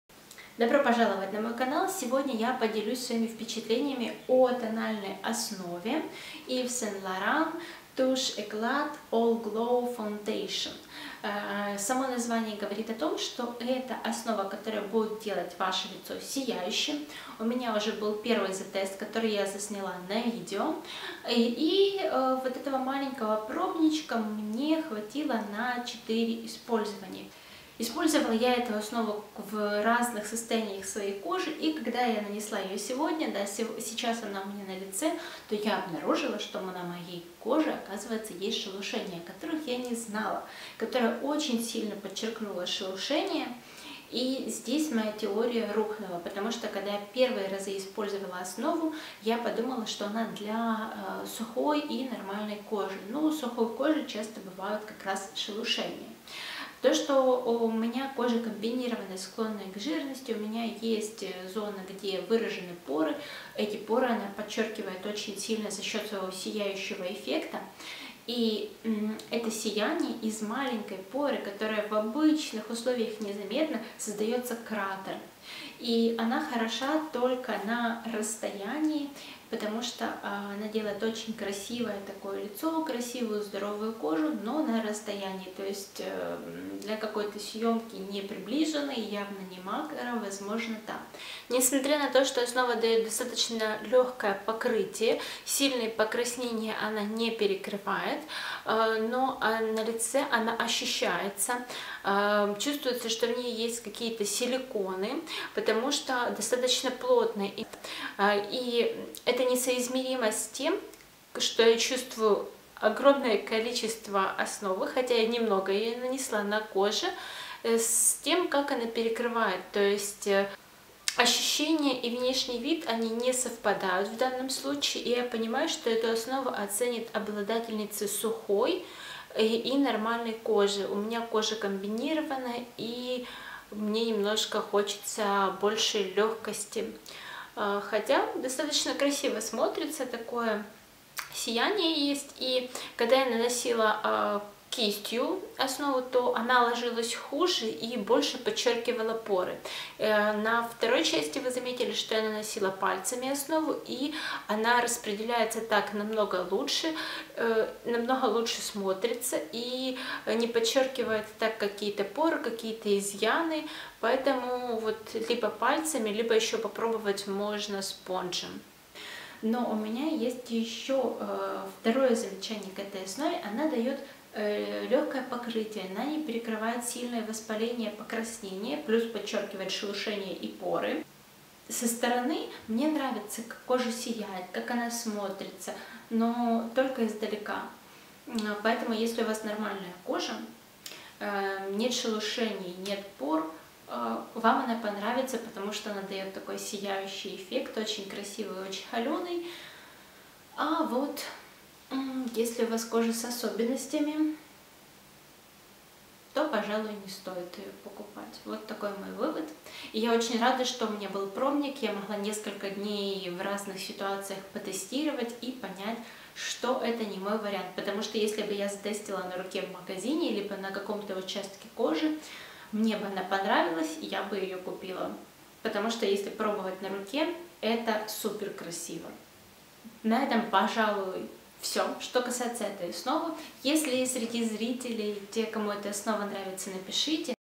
Кому подойдёт основа YSL Touche Eclat All In One Glow
Тональная основа Yves Saint Laurent Touche Eclat All-In-One Glow